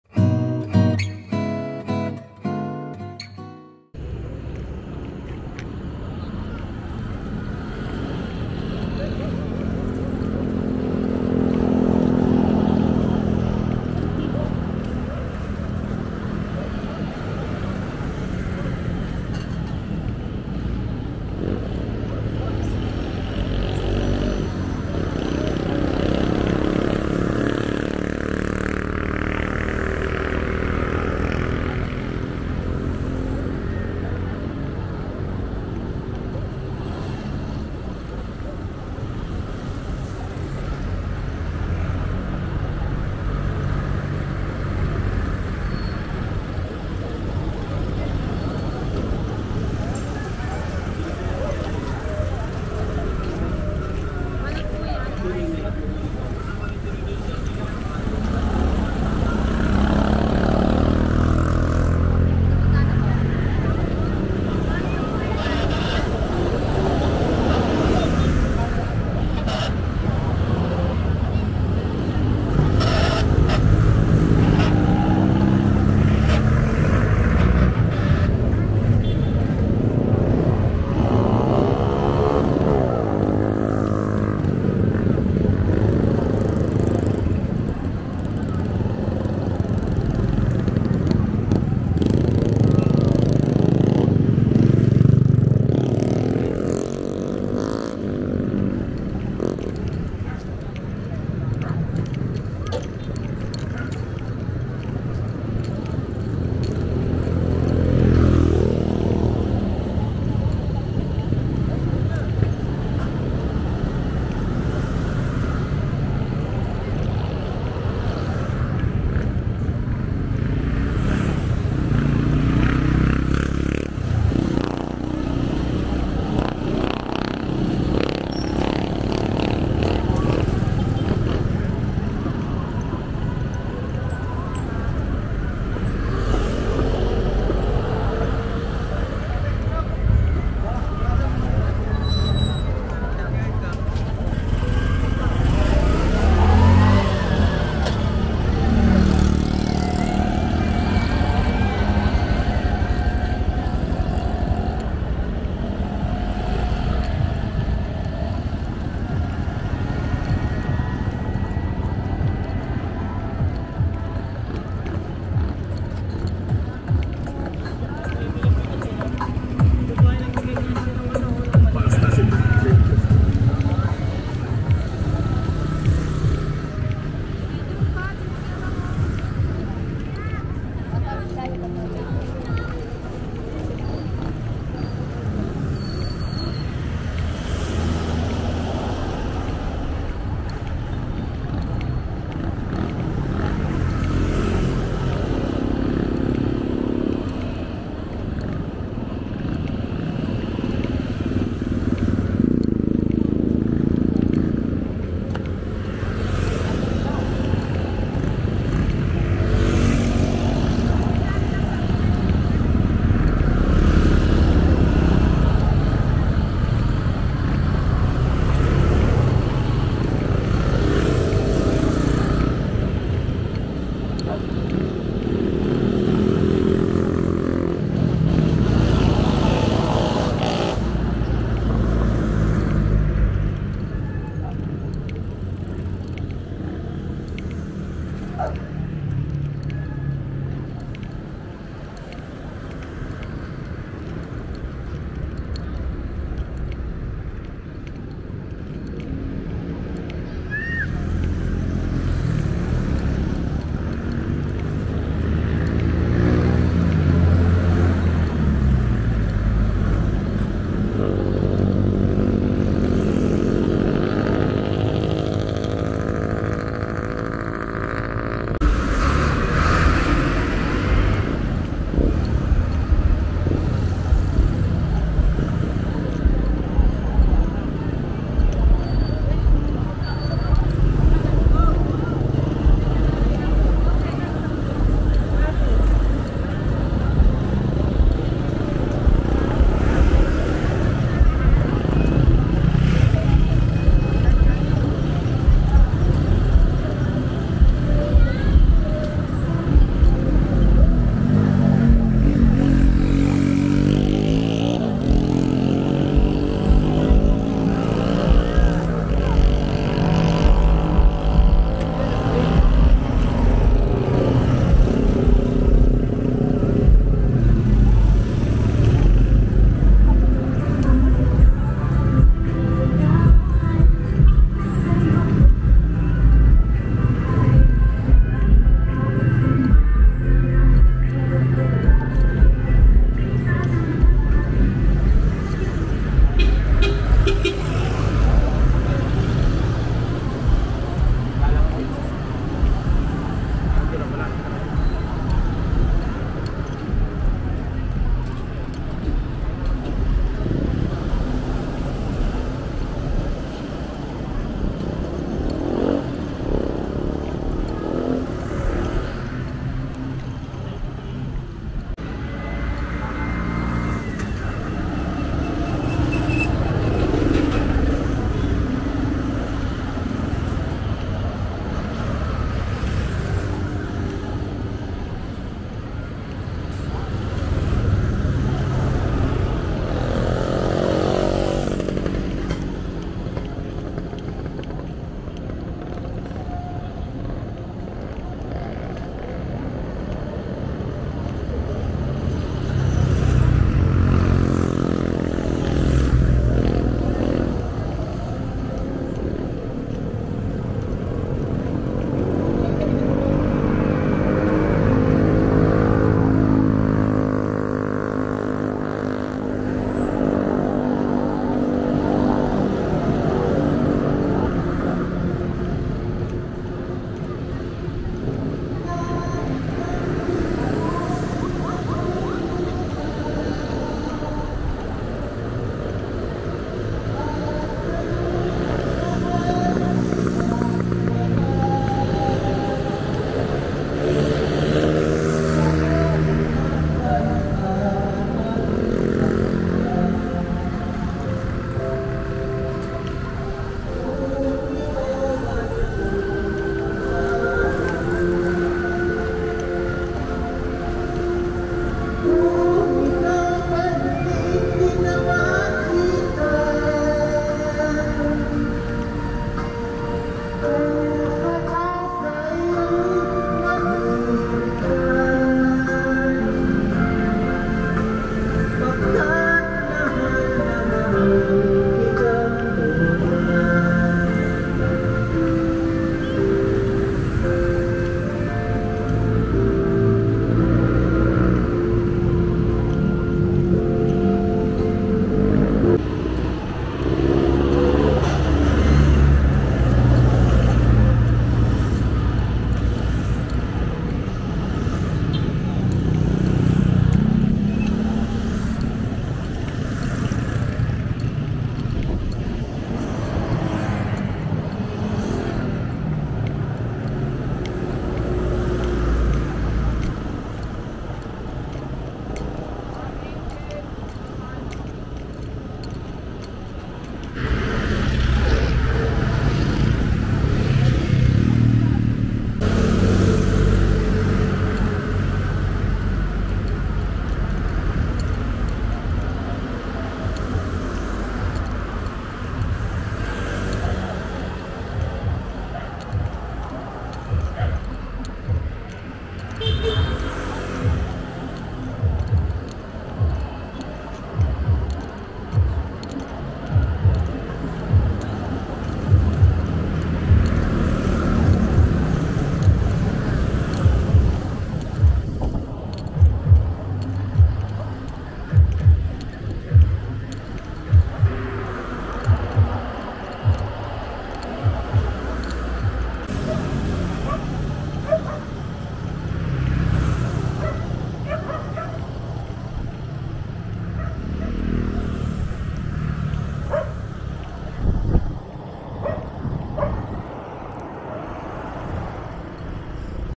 4K | Evening Walk Along Marilaque Highway or Marikina-Infanta Highway Rizal Philippines
See what it looks like here in Marikina-Infanta Highway or known as Marilaque Highway at evening...